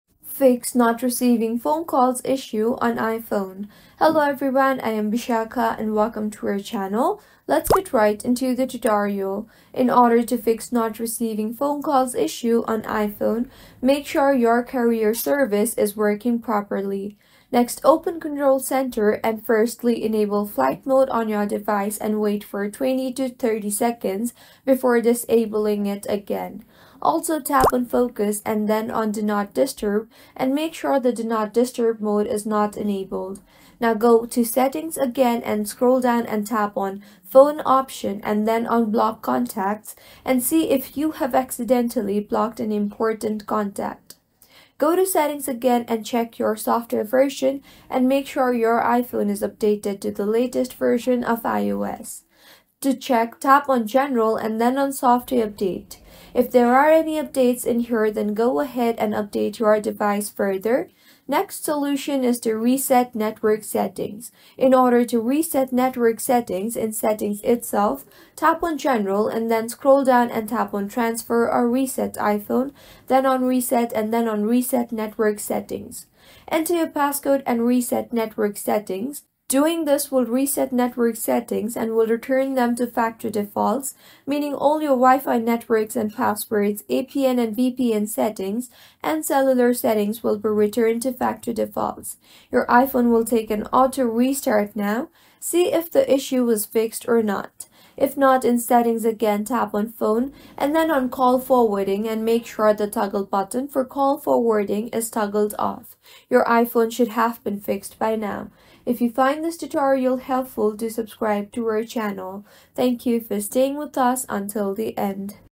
Fix Not Receiving Phone Calls Issue On iPhone 15, 15 Pro, 15 Pro Max
Are you facing the issue of not receiving phone calls on your iPhone 15, 15 Pro, or 15 Pro Max? We've got you covered! In this tutorial, we'll walk you through the steps to fix this problem and ensure your phone calls come through without any hiccups.

Learn about the frustrating issue of not receiving phone calls on your iPhone 15 series devices and why it's important to resolve this issue promptly.

Step-by-Step Tutorial:

[00:00] Introduction and Problem Overview
[00:10] How to Fix Not Receiving Phone Calls Issue on iPhone 15, 15 Pro, 15 Pro Max

In just 10 seconds, we'll provide you with a quick and effective solution to ensure you never miss an important call again.